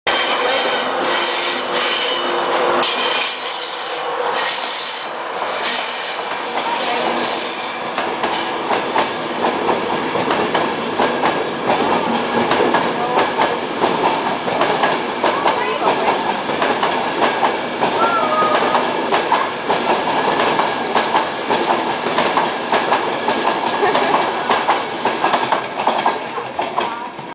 East coast class 91 departs york.
An east coast class 91 departs York for Newcastle on 22.8.13. sorry about my fingers getting in the way.